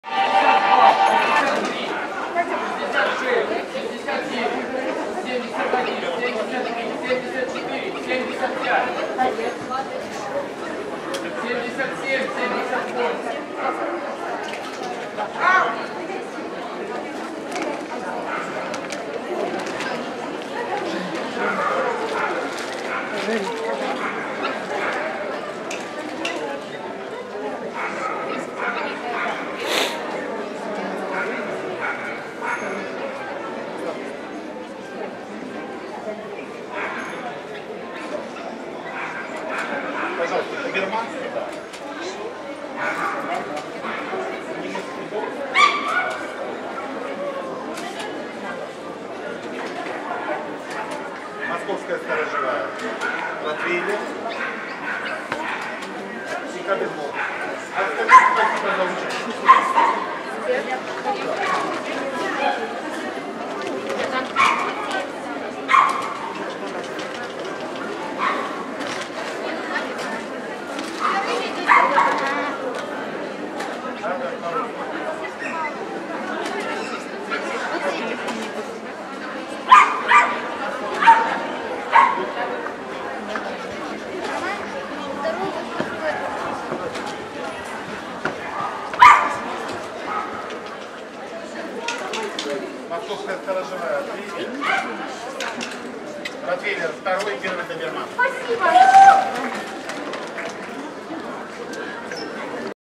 26.12.2020 Белгород, Россия, "Зимний Кубок Белгорода 2020" Бест 2 группы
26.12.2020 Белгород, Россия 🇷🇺
Национальная выставка собак всех пород, ранга САС- ЧФ
"Зимний Кубок Белгорода 2020" ,
Эксперт: Васильев О.
Класс\Чемпионы
Louis Vuitton Dlya Stella Ardens (Босс)🔥💖 -28.5 months old
(Smoke del Casco Viejo & Viva La Diva Di Stella Ardens)
Отлично, САС,ЧФ, ВОВ, ЛК 🏆🥇👍💪